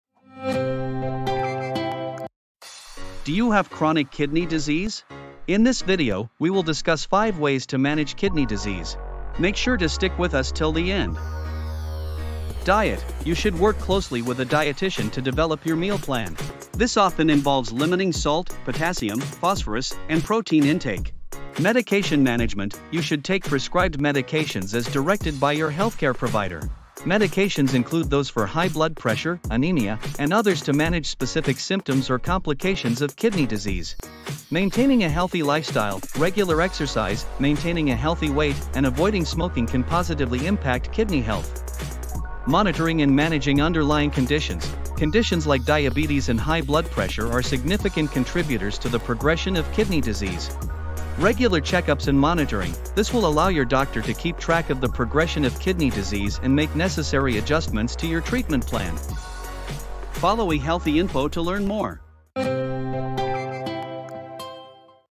Mastering Kidney Disease Management | Essential Tips for Health Kidney
Kidney Disease Management | Management Of CKD Patients | Managing Chronic Kidney Disease | E Healthy Info

Learn essential tips for managing kidney disease effectively! Discover expert advice on diet, medication management, maintaining a healthy lifestyle, monitoring underlying conditions, and the importance of regular check-ups. Watch now to take control of your kidney health!

Timestamps:
0:00 Intro
0:05 5 tips to manage kidney disease
0:12 Tips 1 - Diet
0:21 Tips 2 - Medication Management
0:34 Tips 3 - Maintaining Healthy Lifestyle
0:42 Tips 4 - Monitoring Underlying Conditions
0:51 Tips 5 - Regular Monitoring